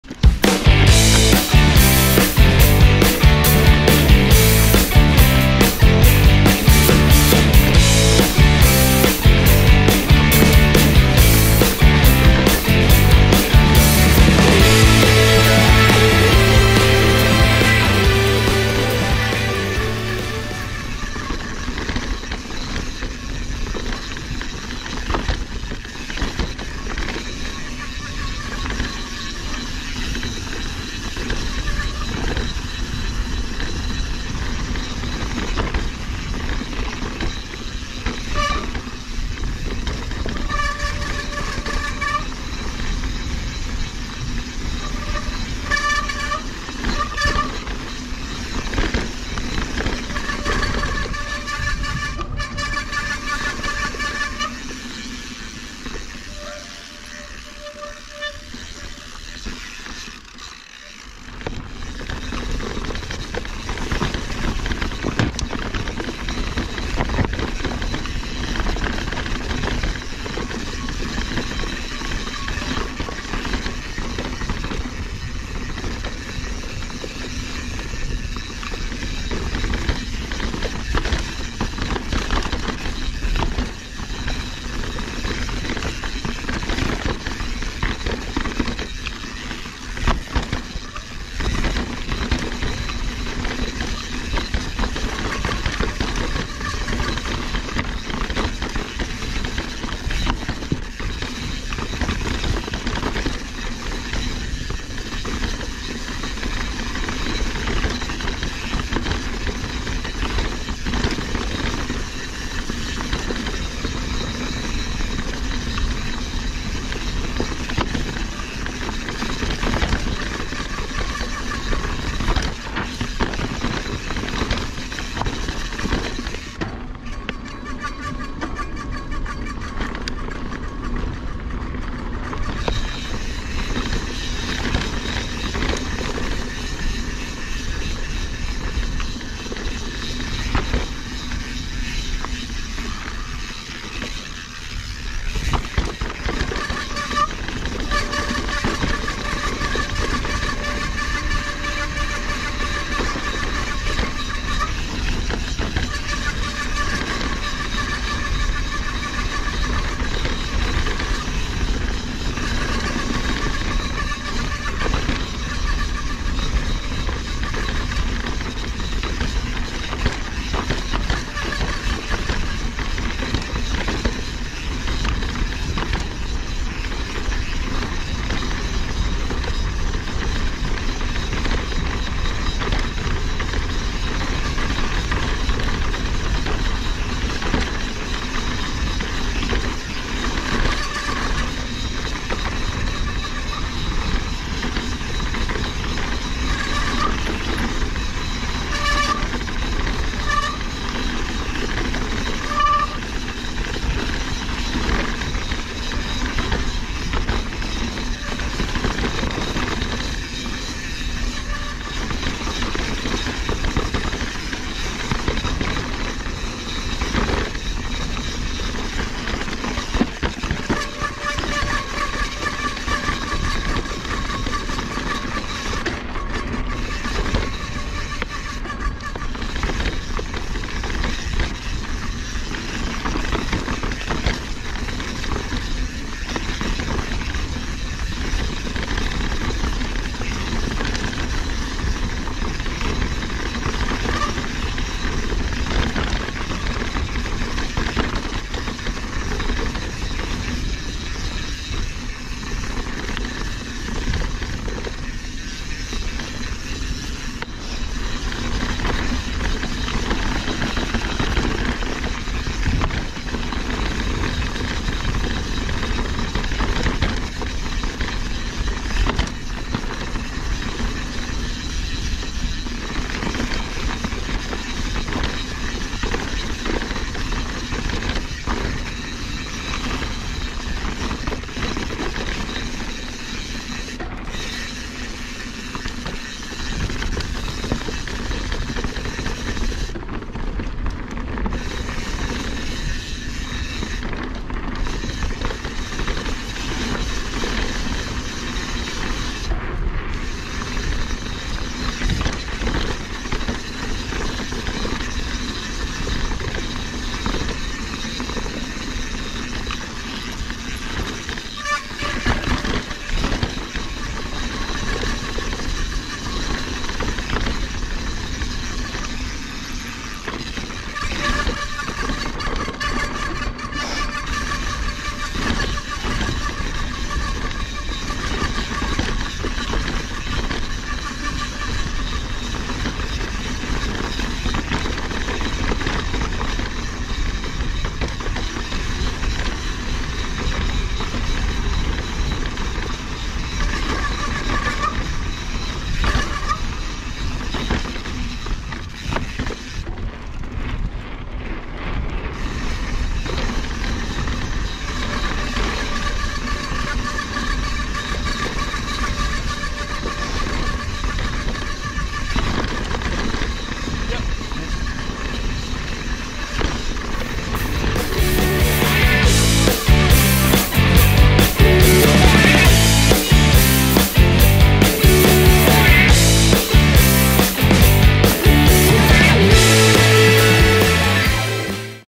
Mountain biking Eagle Eye to Falcon Flow connector trail || Moab, UT
Chunky jeep trail that connects Eagle Eye to Falcon Flow.